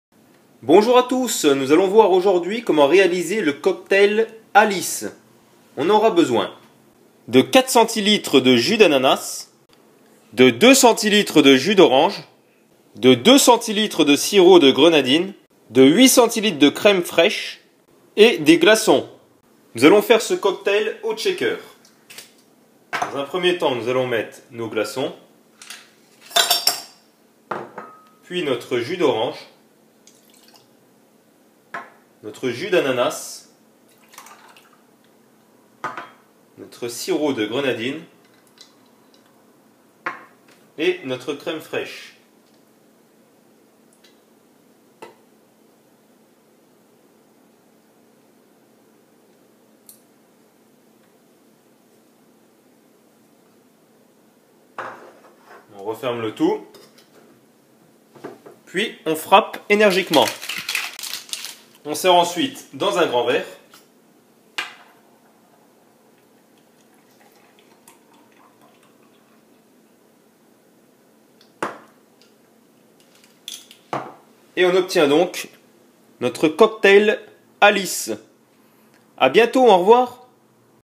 Faire un cocktail Alice - Boisson sans alcool
Pour faire un cocktail Alice, vous avez besoin de jus d'ananas, de jus d'orange, de sirop de grenadine, de crême fraîche et de glacons.

Retrouvez nos autres vidéos tutoriels, trucs et astuces pour tout apprendre de la cuisine sur notre chaîne